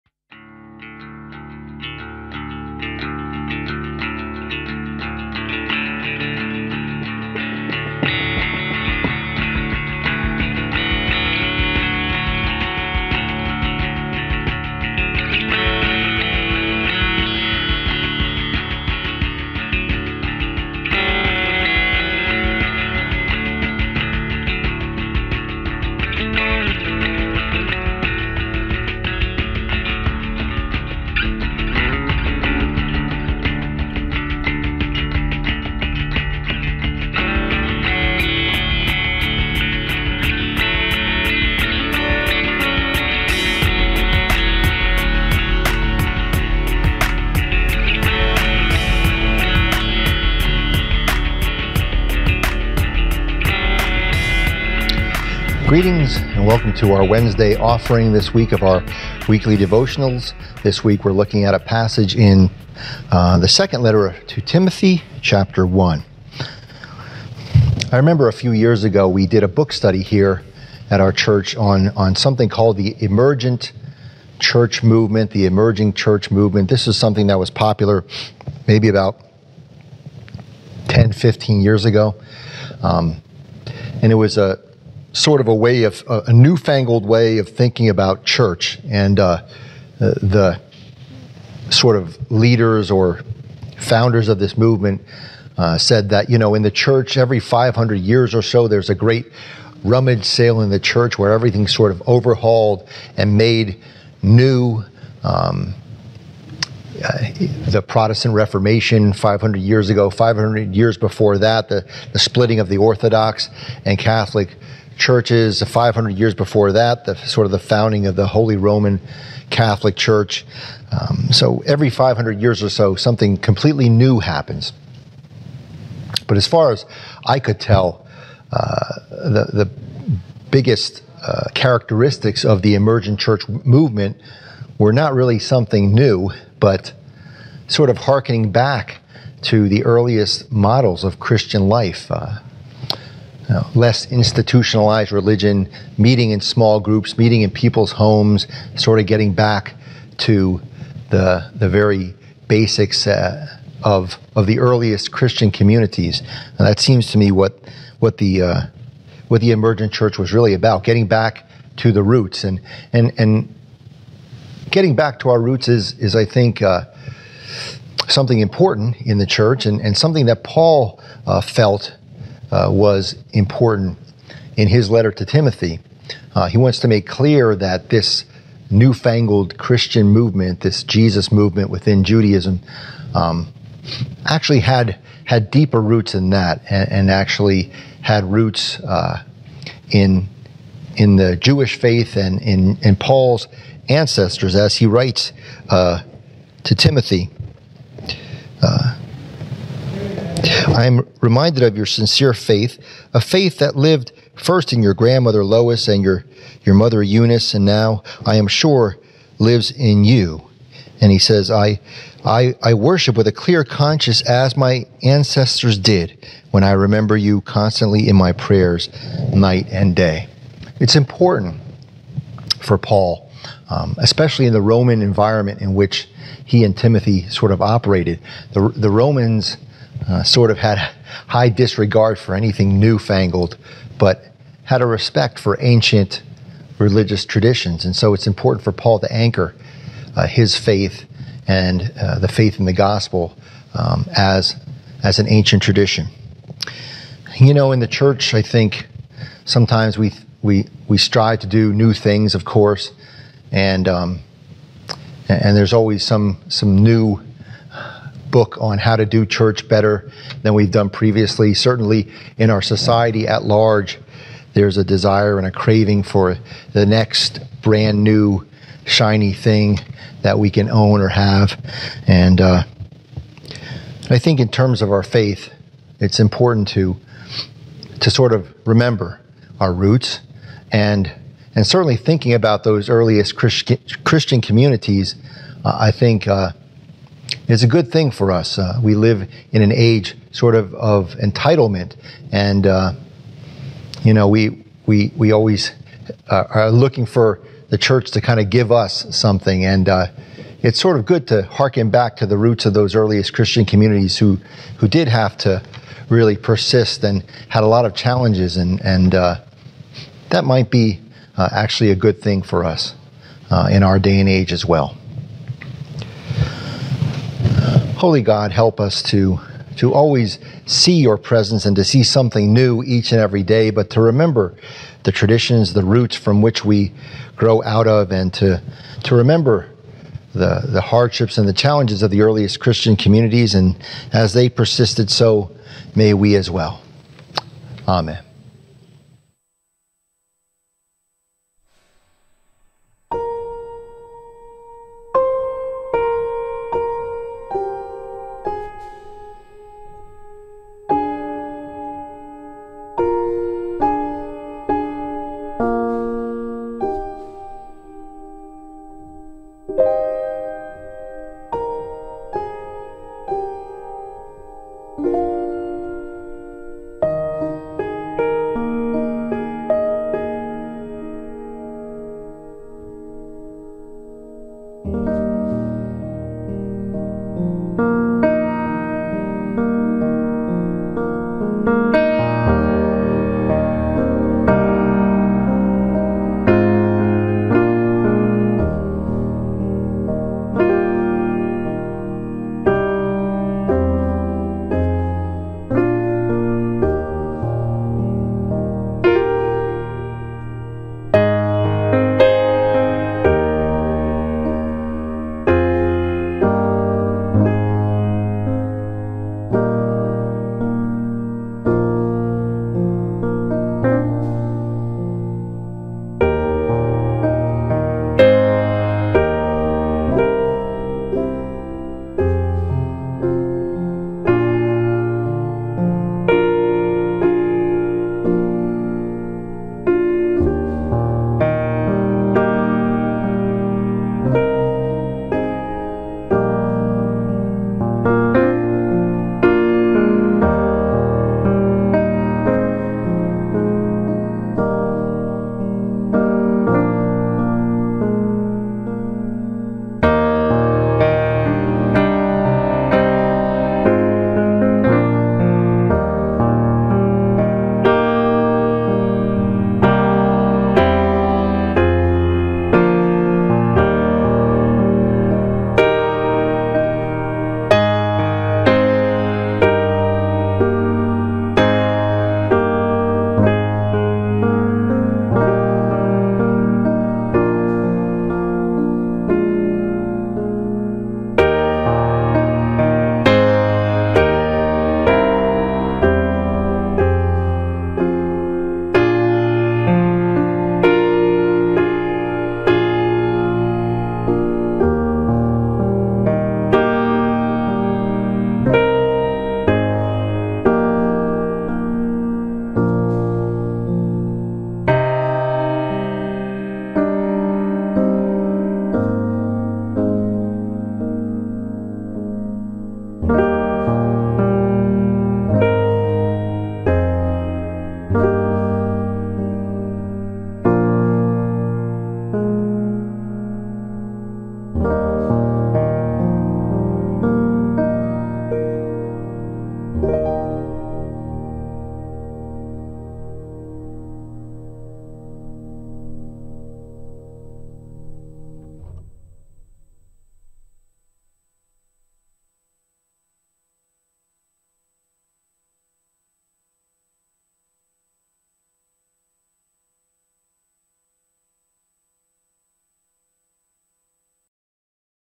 Forks United Church of Christ Daily Devotional - September 28, 2022
Forks UCC hopes to care for your heart, mind, and soul and give you strength with daily devotionals.  Presented by Reverend Robert Gutekunst, Music by Anthony Newett, Produced by Newett Studios,